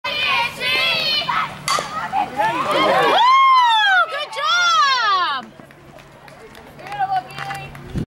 Thrashers 9-23-2018 Kiele hit
Nice shot up the center Kiele, way to go!